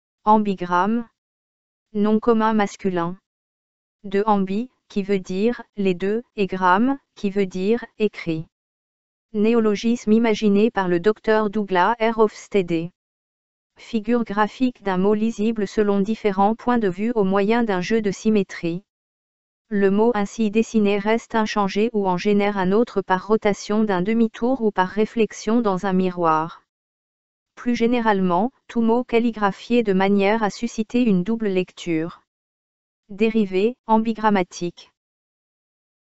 Ambigramme | Dictionnaire français audio
Définition du terme Ambigramme.
De Ambi-, (qui veut dire : « les deux »), et -gramme, (qui veut dire : « écrit »). Néologisme imaginé par le Dr. Douglas R. Hoffstader.
Figure graphique d'un mot lisible selon différents points de vue au moyen d'un jeu de symétrie. Le mot ainsi dessiné reste inchangé ou en génère un autre par rotation d'un demi-tour ou par réflexion dans un miroir. Plus généralement, tout mot calligraphié de manière à susciter une double lecture.
Dérivé : ambigrammatique.